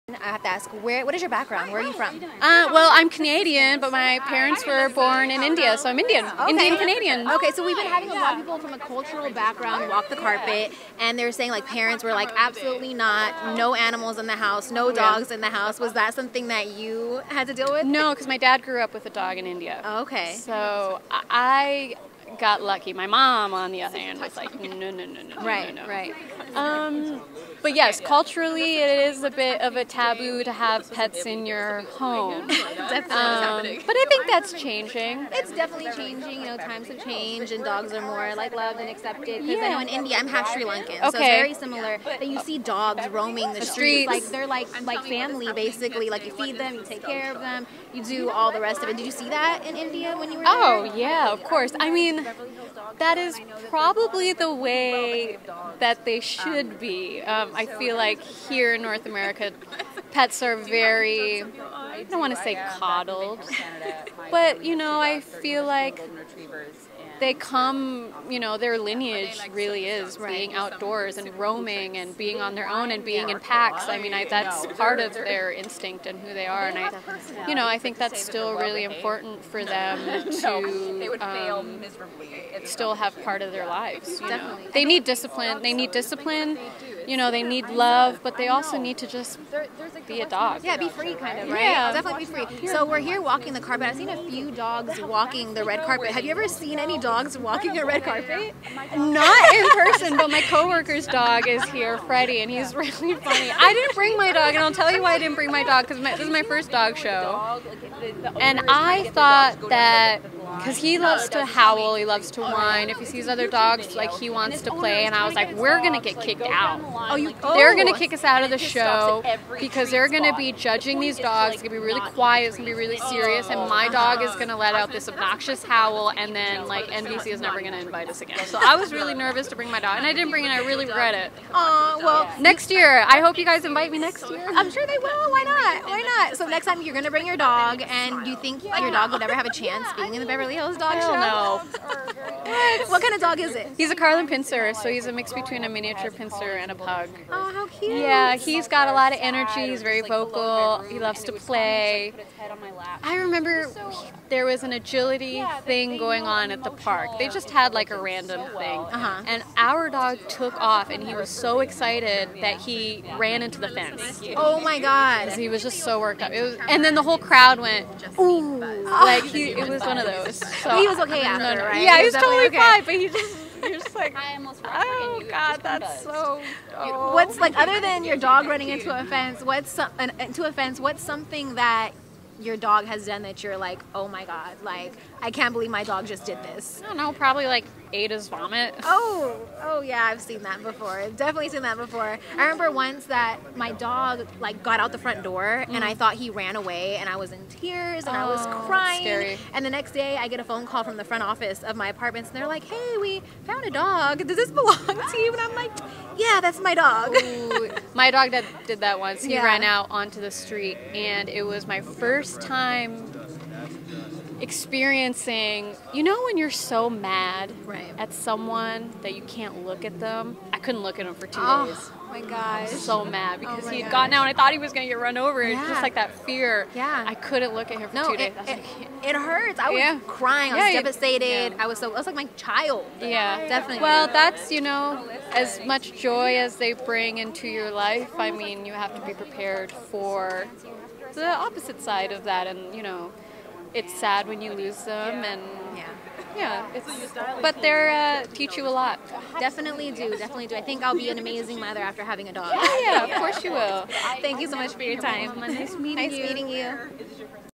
Parveen Kaur Speaks On Manifest, Indian Culture, and Dogs
Parveen Kaur attends the Beverly Hills Dog Show